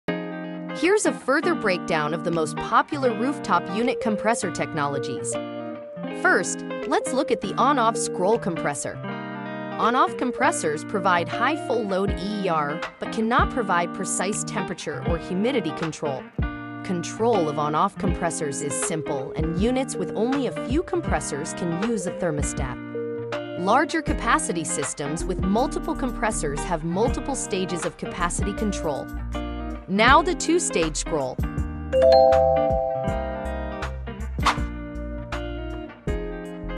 Rooftop Unit Compressor Technology | On/Off Scroll Compressors, Pros and Cons
Today, we are lucky to have multiple compressor options for packaged rooftop units.

On/off compressors, two-stage, digital compressors, and variable-speed compressors.

The common question is, which compressor should I use and why?

Please listen as Tonya introduces our multipart series reviewing current packaged rooftop unit compressor technology.

There are engineering tradeoffs to all HVAC technology. When looking at packaged RTUs, the type of compressor we choose should be based on the following five factors: part load efficiency, full load efficiency, first cost, the complexity of controls, and precision of comfort control.

This information comes from the AAON Rooftop Unit Compressor Technologies Guide.

This compressor technologies guide gives an overview of the benefits and costs of different compressor configurations and the efficiencies possible with different configurations of rooftop units. AAON recognizes many reasons for choosing a specific compressor technology and offers many options to meet a specific application’s needs.